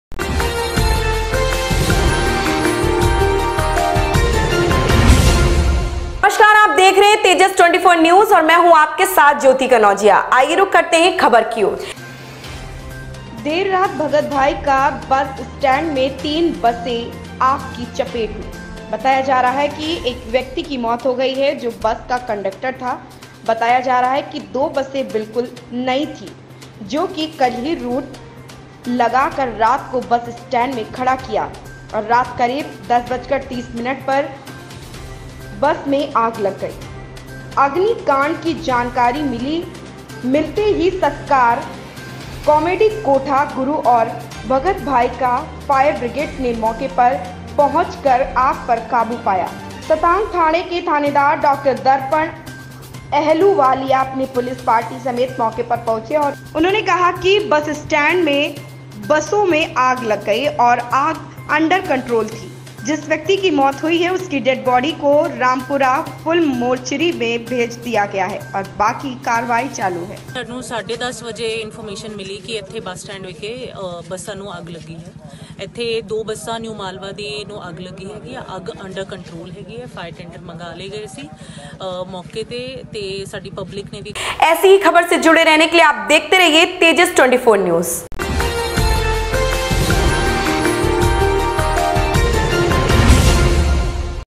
Bathinda के Bus Stand में लगी भीषण आग,3 बसें पूरी तरह जलकर राख | Hindi News
देर रात भगत:   भाई का  के बस स्टैंड में 3 बसें को आग लग गई  बताया जा रहा के एक व्यक्ति की मौत हो गई जो बस का कंडक्टर था बताया जा   रहा है के 2 बसें बिलकुल नई थी जो के कल ही 2 रूट लगा कर रात को बस स्टैंड में लगा दी और रात करीब 10:30 बजे बसें को आग लग गई  अग्नि कांड की जानकारी मिले ही सत्कार कॉमेडी कोठा गुरु और भगत भाई का की फायर ब्रिगेड ने मोके पर पोहंच कर आग पर काबू पाया stahank ठाणे के थानेदार डॉ. दर्पण अहलूवालिया अपनी पुलिस पार्टी समेत मोके पर पहुंचे और उन्होन कहा की बस स्टैंड में बसें को आग लग गई और आग अंडरकंट्रोल थी   जिस व्यक्ति की मौत हुई उसकी डेड बॉडी को रामपुरा फूल मोर्चारी भेज  दिया गया है और बाकी करवाई चालू है